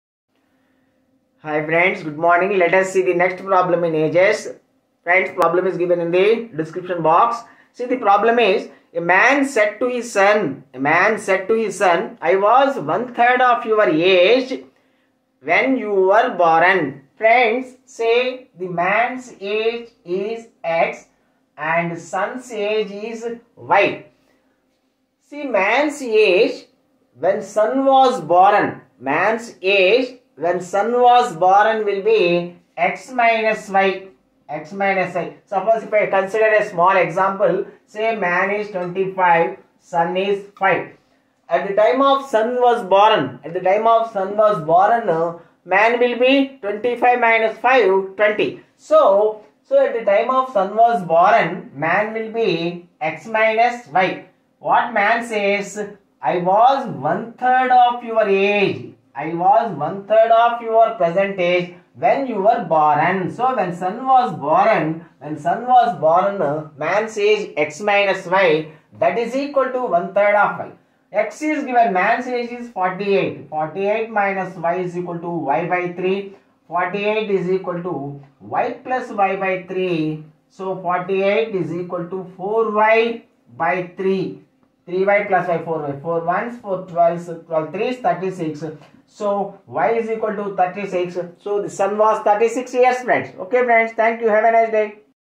A man said to his son, "I was one-third of your present age when you were born". If the present age
A man said to his son, "I was one-third of your present age when you were born". If the present age of the man is 48 years, find the present age of the son.